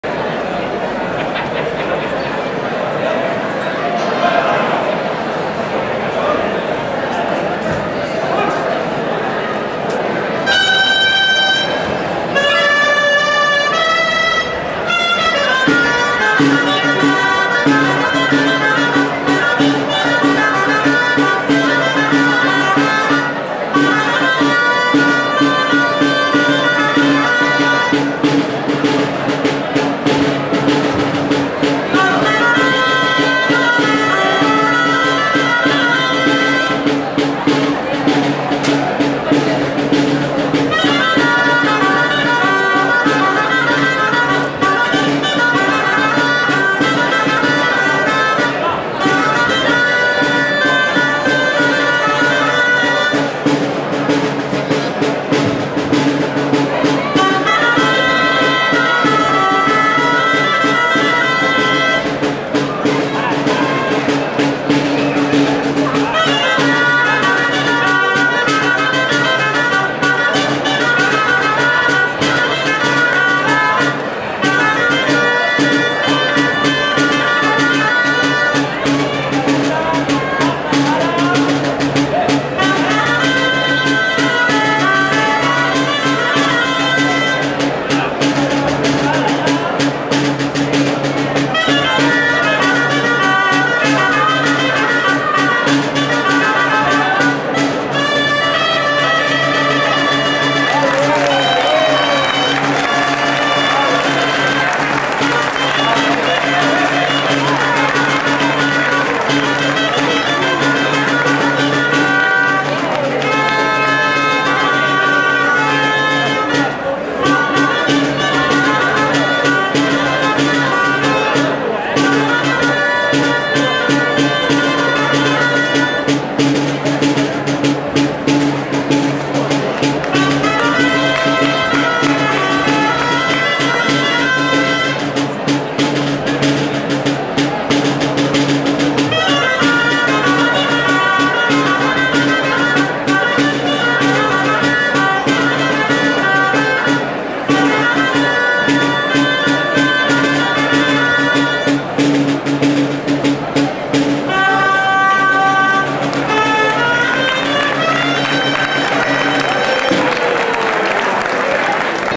3de7 32è Aniversari dels Bordegassos de Vilanova 2006
Tres de set descarregat al 32è Aniversari dels Bordegassos de Vilanova, celebrat a la Vilanova i la Geltrú, el diumenge 23 d’Abril de 2006